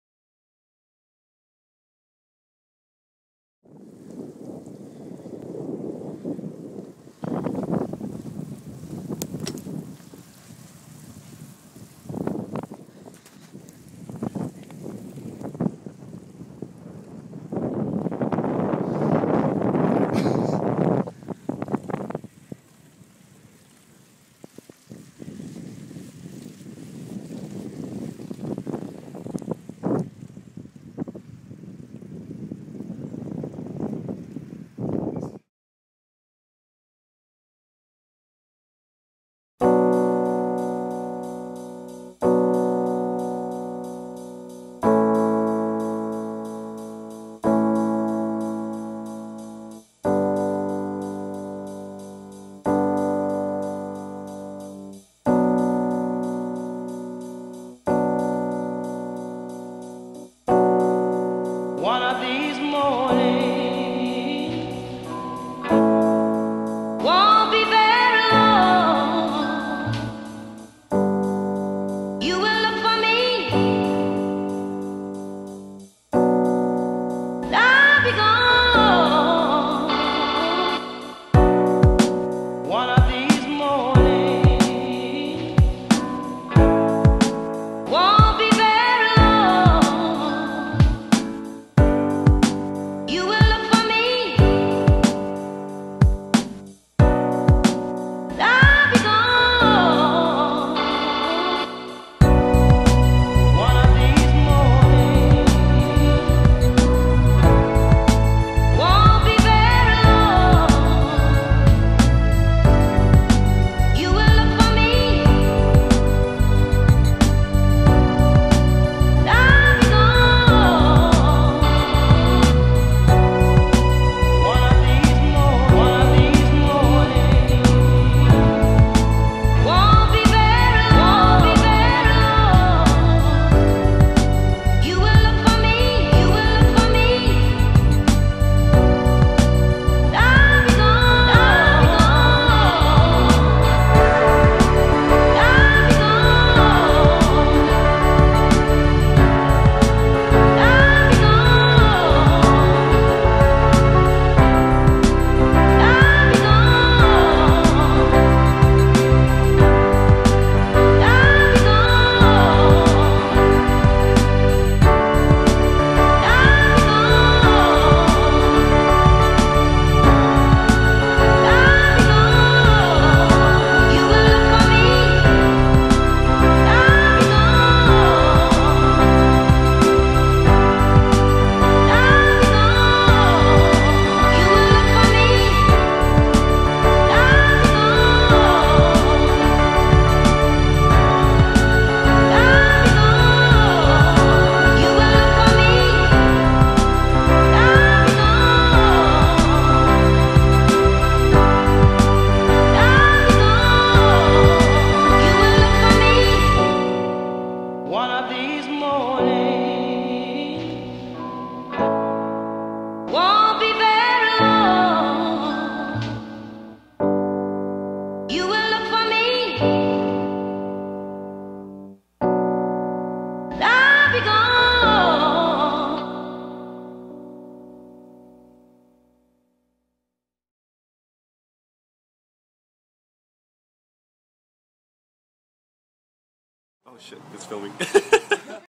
Bicycle Touring Pacific Coast Highway
Slideshow of Pablo's solo bike touring trip from Seattle, WA to Santa Cruz, CA